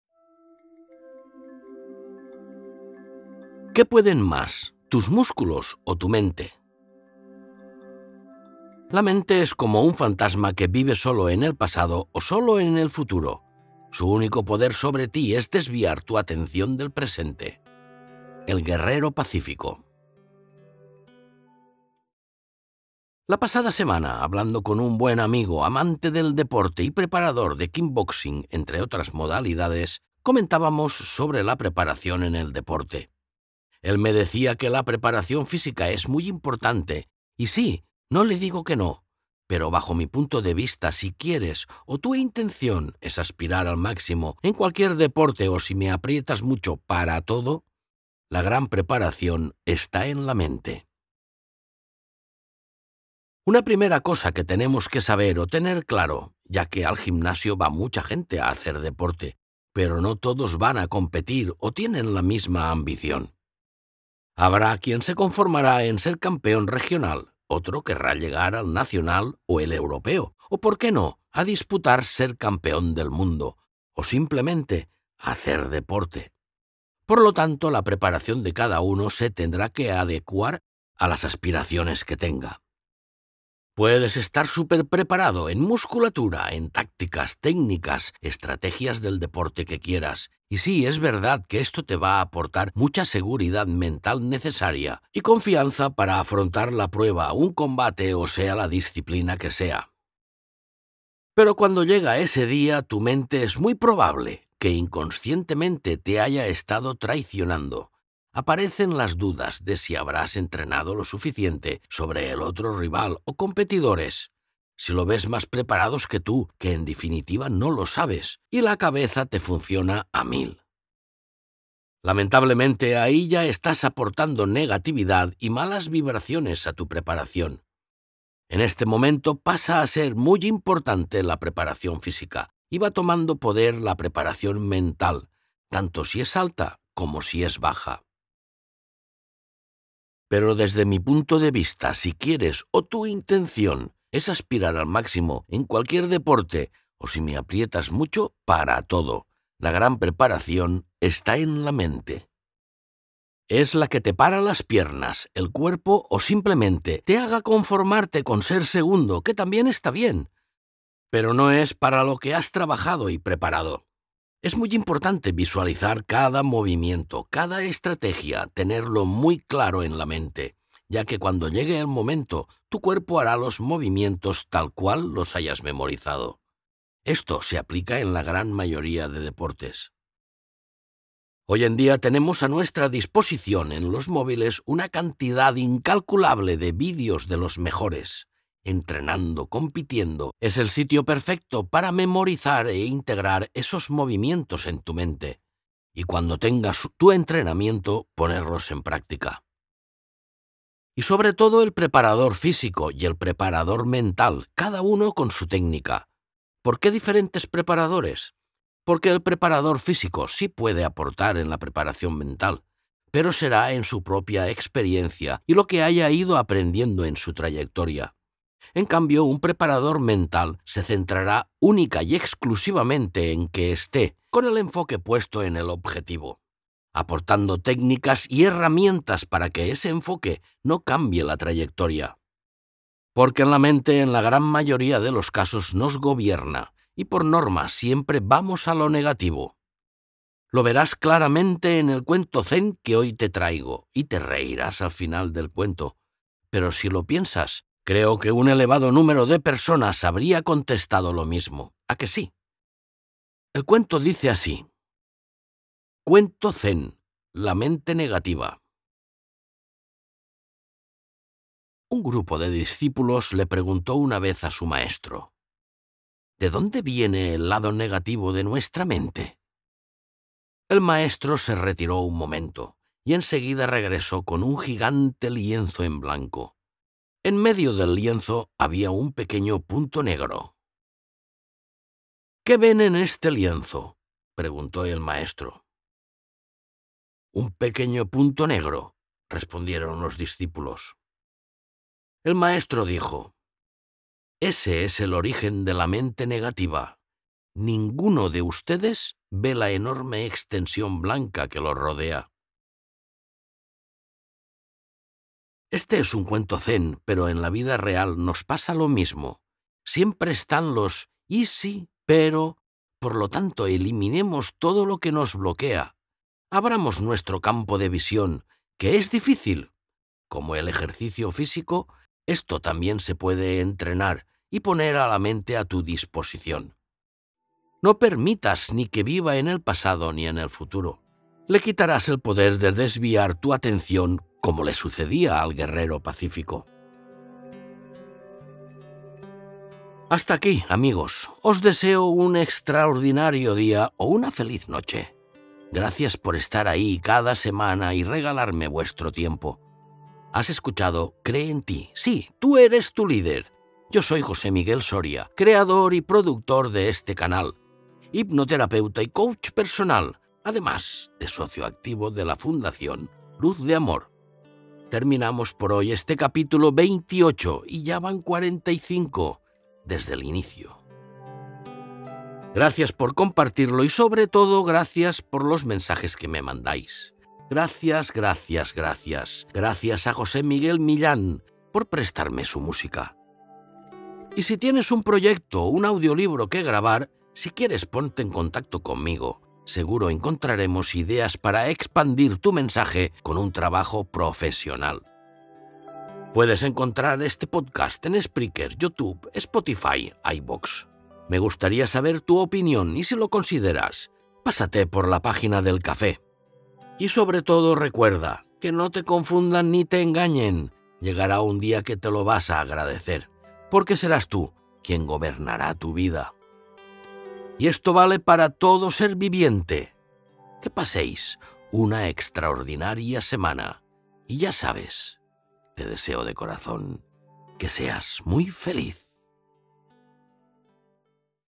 Músculos o mente
Es importante la preparación física para cualquier deporte, pero no olvidemos la importancia de la preparación mental para que no te traiciones.

Te explico con un cuento Zen

Espero que te guste y ya sabes...

Te deseo de corazón…

¡¡¡Que seas muy feliz!!!

Jose Miguel